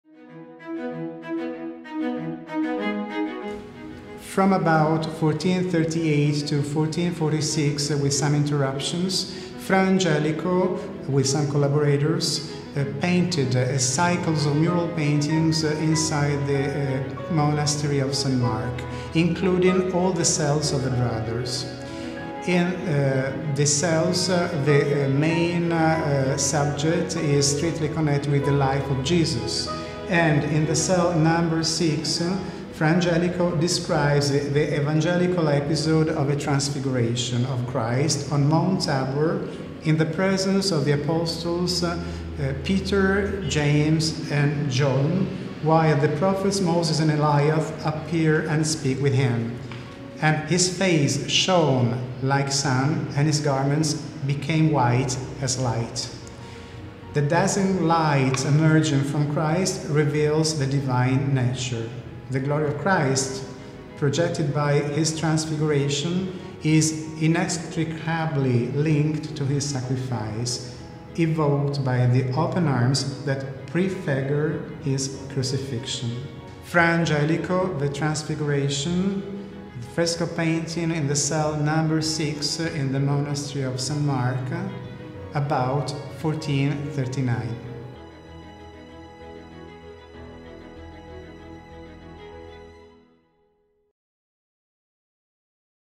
Menarini Pills of Art: Die Verspottung von Cristh von Fra Angelico (Untertitel Deutsch)
Entdecken Sie mit uns das Kloster San Marco voller Fresken von Fra Angelico. In Zelle Nr. 6 steht The Mocking of Cristh mit seinen offenen Armen und erinnert an die Kreuzigung des Werkes von Cristh von etwa 1439.